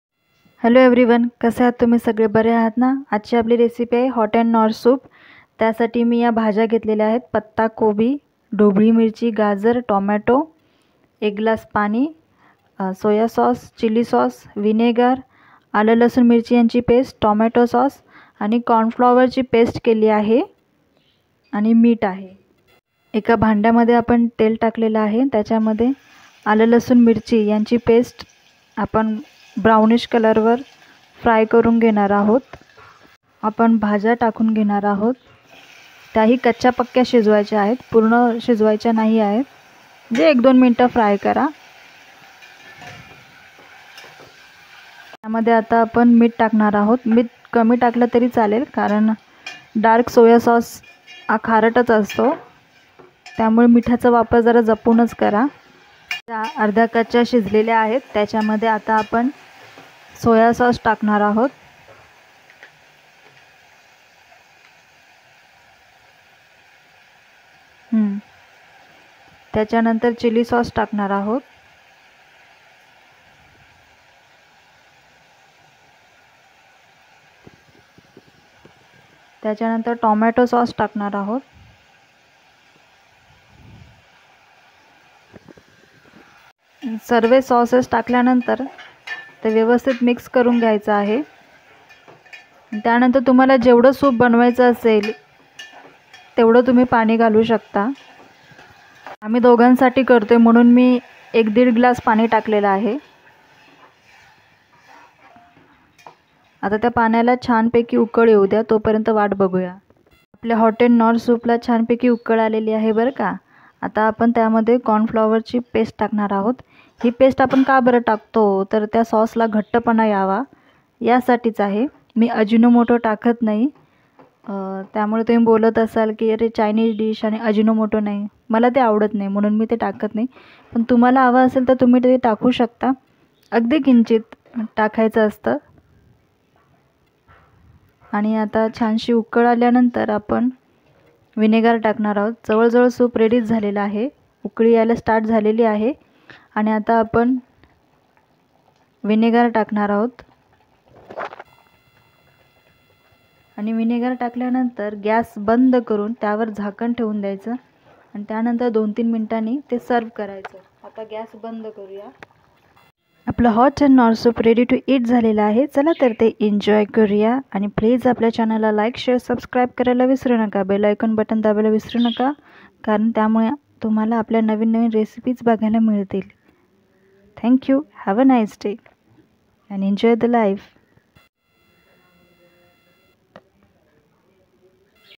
hot & nour soup
पता कोबी, ढोबळी मिरची, गाजर, टमाटर लांब आणि पातळ कापून घ्या.
सोया, chilly, टोमॅटो sous, vinegar,
मीठ, पाणी आवश्यकतानुसार
वाटणं :- आले, लसूण, मिरची